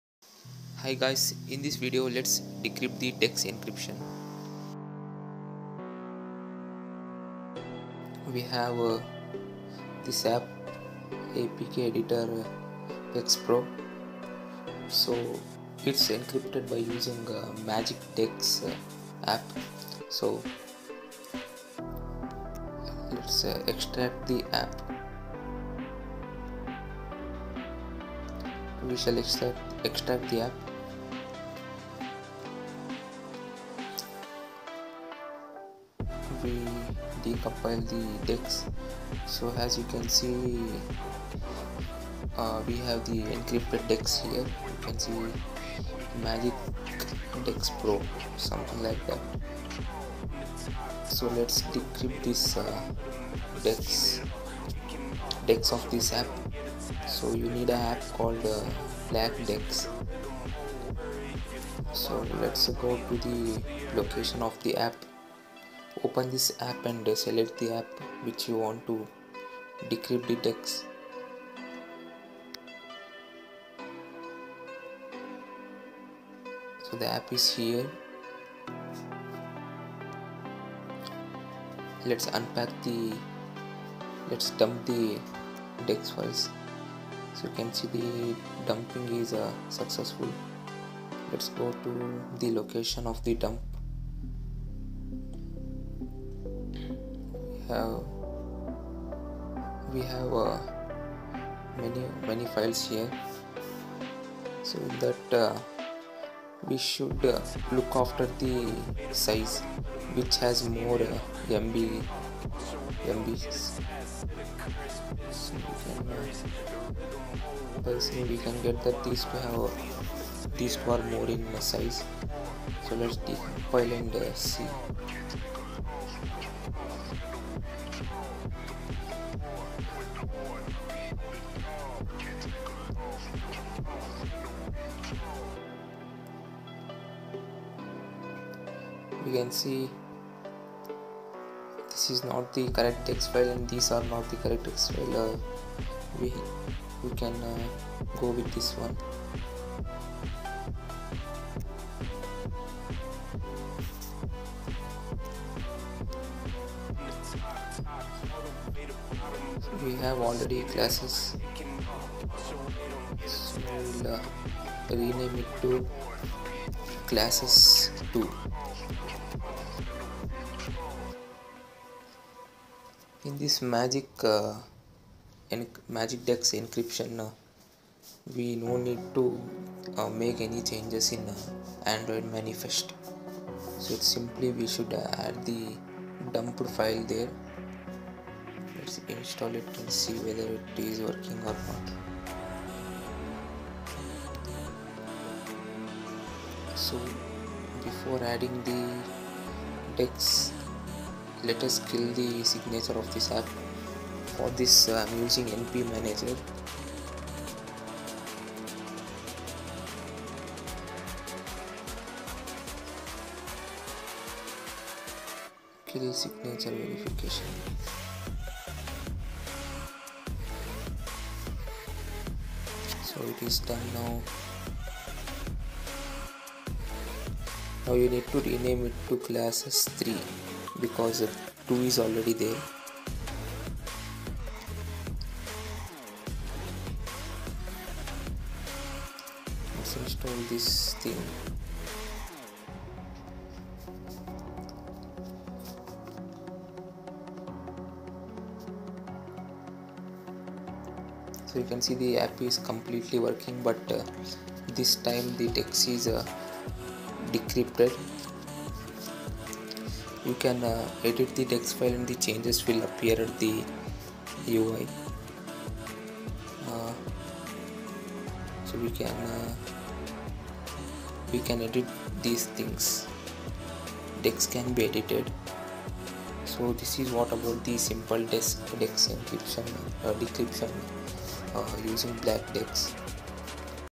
How To Dump Dex Without Root
THIS VIDEOS WILL HELP YOU IN YOUR GAME MODDING JOURNEY

🔓 DEX Decryption Without Root | Dump .dex Easily (No Root Required!)

In this video, I’ll show you how to decrypt and dump DEX files from Android apps without root access — using some powerful tools:

✅ MT Manager
✅ NP Manager
✅ BlackDex
✅ APK Editor

This is a step-by-step beginner-friendly tutorial perfect for reverse engineers, ethical hackers, or anyone curious about Android internals and app modification.

📲 No root? No problem. Learn how to extract .dex files directly from any APK and view hidden code in minutes.

📌 Tools Covered:

MT Manager: File explorer + hex viewer + smali editor.

NP Manager: Lightweight app control with built-in DEX extraction.

BlackDex: For dumping in-memory decrypted DEX files.

APK Editor: Basic app structure editing and inspections.